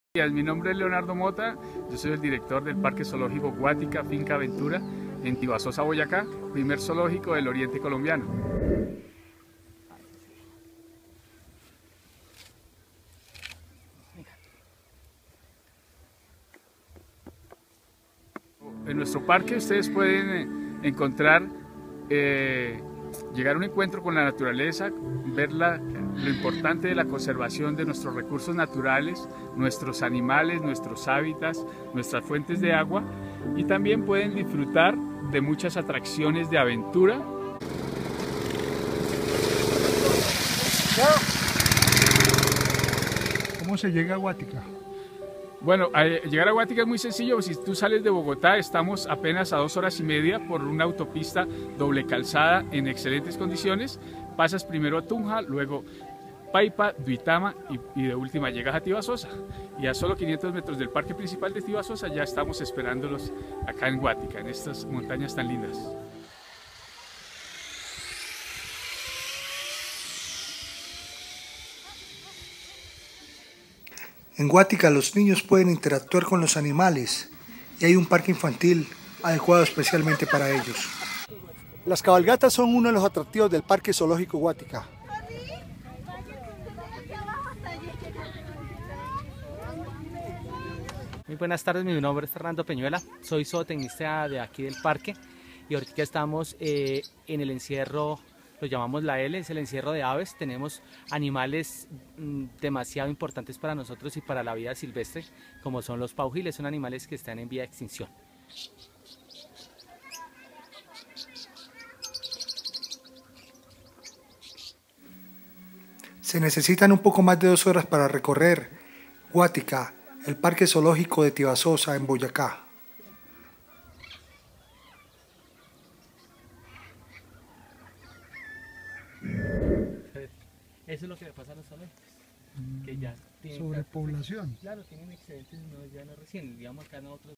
Guatika Zoológico
En Tibasosa nace el nuevo parque zoológica de Colombia Guatika.